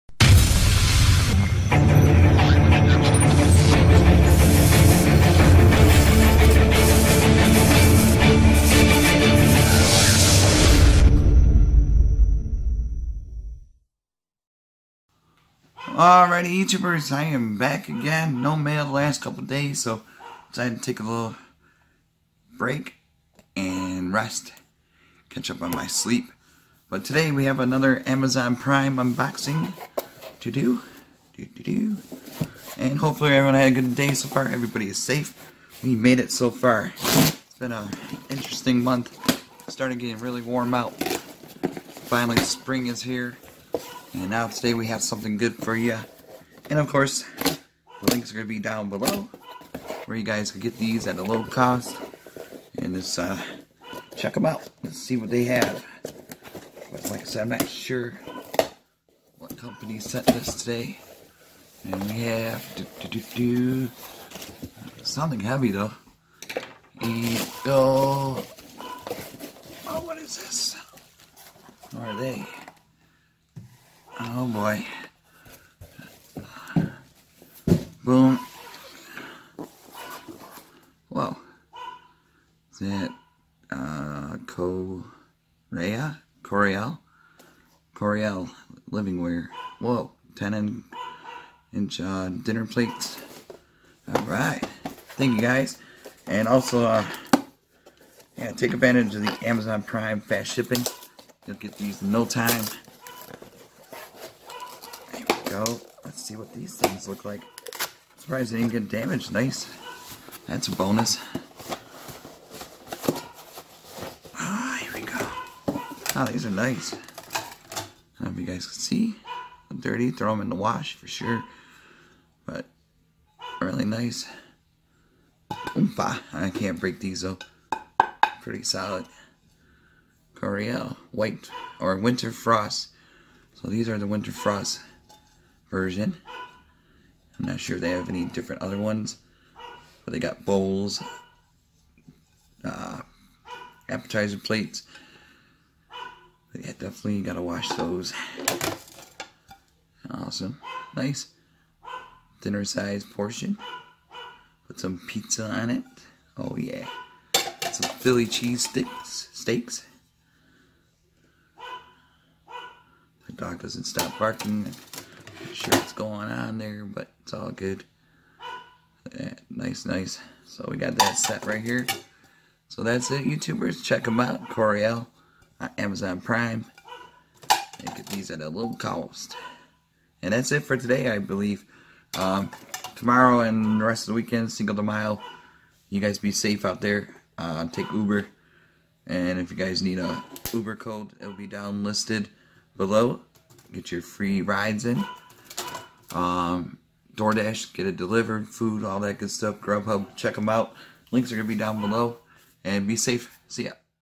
(Episode 2039) Amazon Prime Unboxing: Corelle Winter Frost 6-Pack Lunch Plates
MORE GOODIES

30days free amazon music

2 FREE audible books Amazon Kindle

(MSTOUT2AC150)

10 dollars off YummyBazzaar